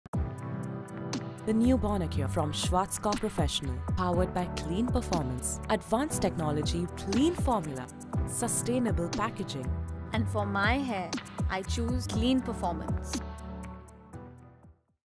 New BONACURE | Clean performance & advanced hair care | Mira Kapoor | By Schwarzkopf Professional
Hair experts’ favorite professional hair care brand- Bonacure  is now Clean, and all about giving high performing products without compromise.

Powered by vegan care complex and cell equalizer technology  that are free from sulphates, parabens, mineral oils, phthalates, microplastics, and artificial colourants, Bonacure gives you moisturized, volume filled, healthy hair.  Advanced technologies with vegan keratin that targets hair concerns like strength hydration, frizz control, colour protection, volume boost, mature hair, and pollution.

All the products come in sustainable packaging made out of 97% recycled plastic with 100% recyclable products.

Discover the New Bonacure Clean Performance.